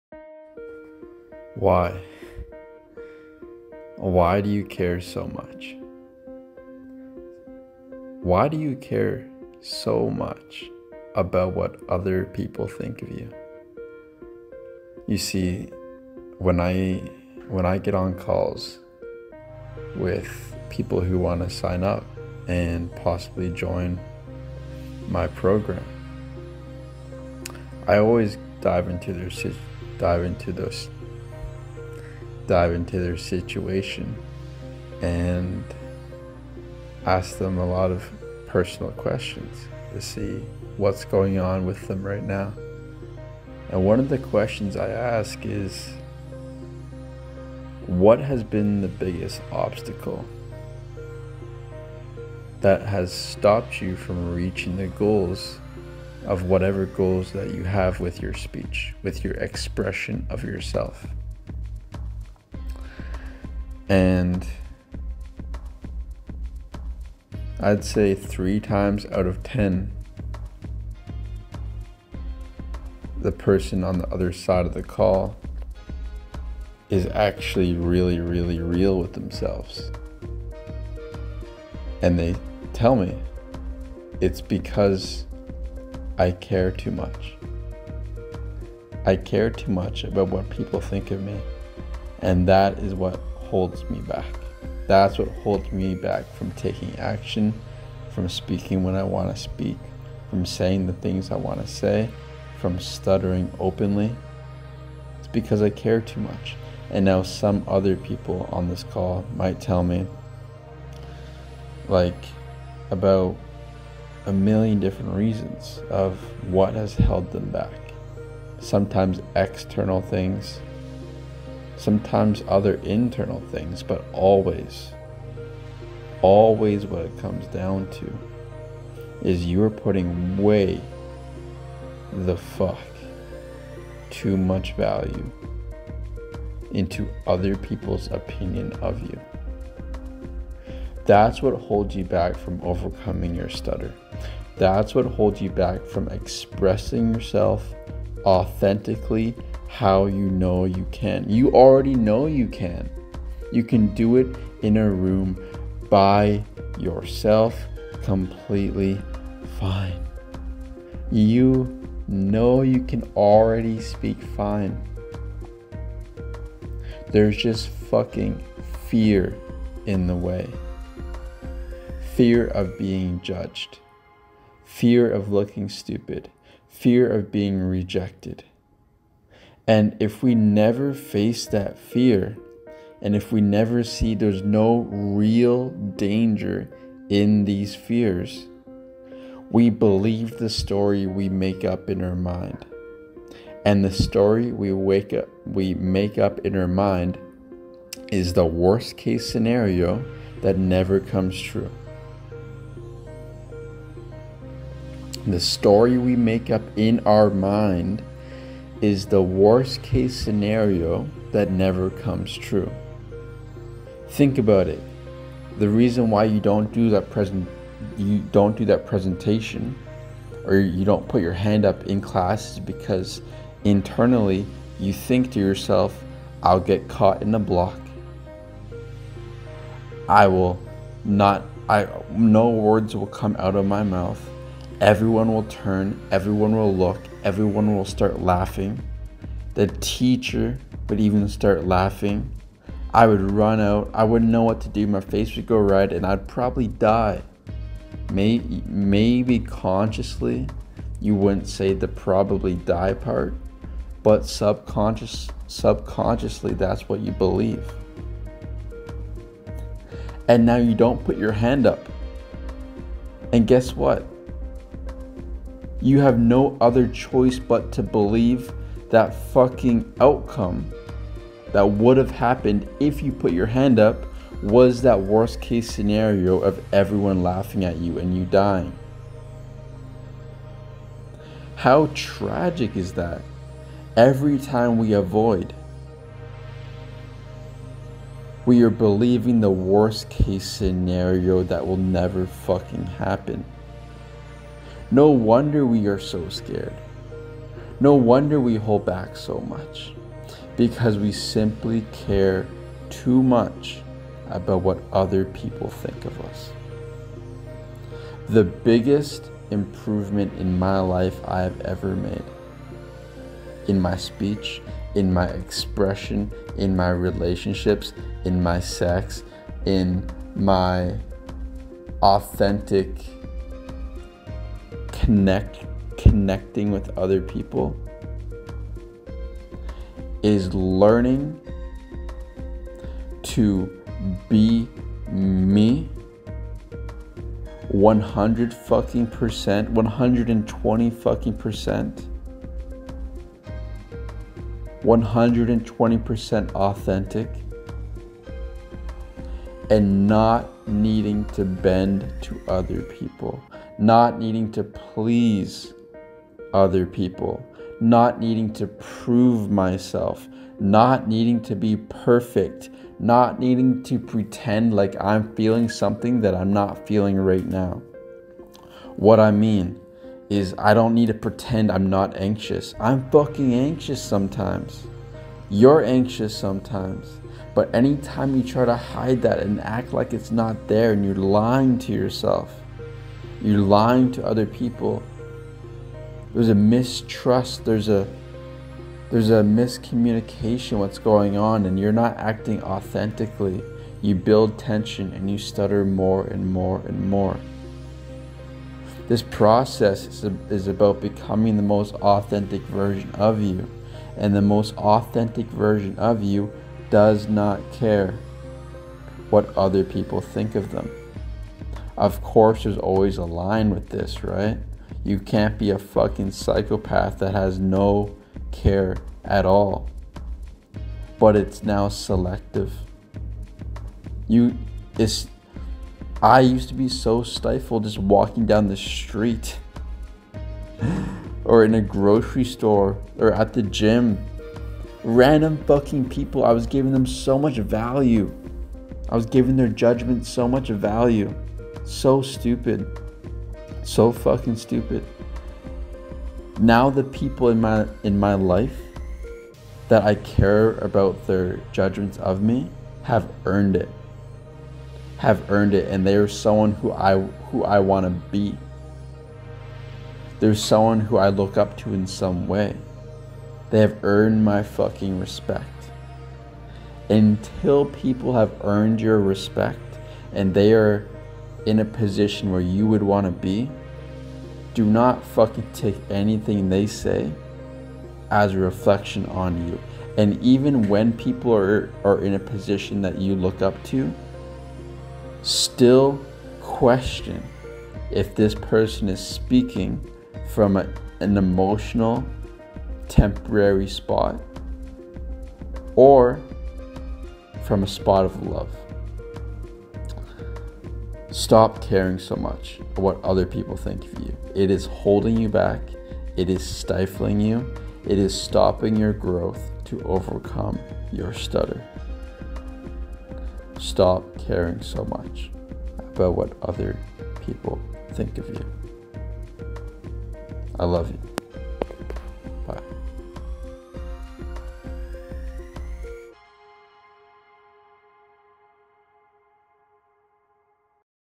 Dear Stutterer: Why Do You Care So Much?
Join my Facebook Group to interact with me and see behind the scenes content

I get this question a lot.. “Chase, what techniques do you use to overcome your stutter?”
The truth is, I would NEVER give you a “speech fluency technique” for you to stop stuttering. WHY? Because these techniques never address the root problem of why you are ACTUALLY stuttering. 99% of the time these techniques only serve as a crutch because your relationship with stuttering is so negative you will do anything to cover it up.

So how can I help you? I can help you if you are ready so forgive techniques and are ready to dive into the new world of authenticity. All your life you have been hiding. Hiding your true personality, hiding from environments, hiding from judgements, hiding from social situations. Overcoming stuttering is about no longer hiding. No longer holding back. Expressing your TRUE AUTHENTIC self that you have been starving the world and yourself from enjoying. When we work together I lead you down the system that has gotten myself and my clients from being scared and isolated to being fearless and authentic.

The world needs to see the real you. You have so much to give. You are funny. You are talented. You are articulate. You are expressive. You are charming. You are attractive. Let's uncover all that is blocking yourself from becoming that person you know you are.